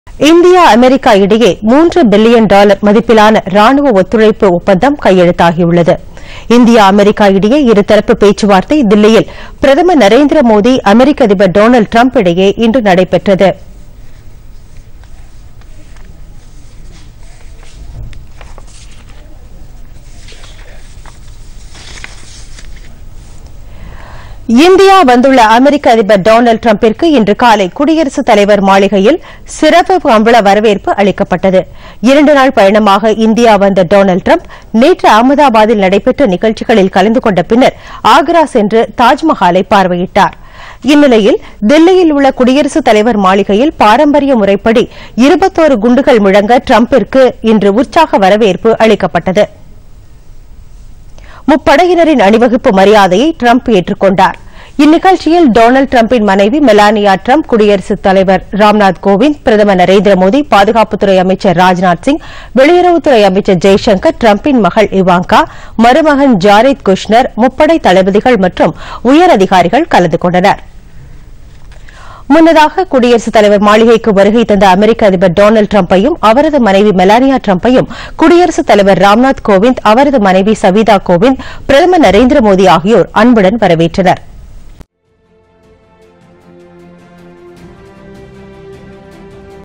ராணுவ ஒப்பந்தம் கையெழுத்து
இந்தியா-அமெரிக்கா இடையே மூன்று பில்லியன் டாலர் மதிப்பிலான ராணுவ ஒப்பந்தம் - பிரதமர் நரேந்திர மோதி - அமெரிக்க அதிபர் டொனால்டு டிரம்ப் முன்னிலையில் கையெழுத்து,
** டொனால்டு டிரம்புக்கு குடியரசுத் தலைவர் மாளிகையில் சிவப்பு கம்பள வரவேற்பு.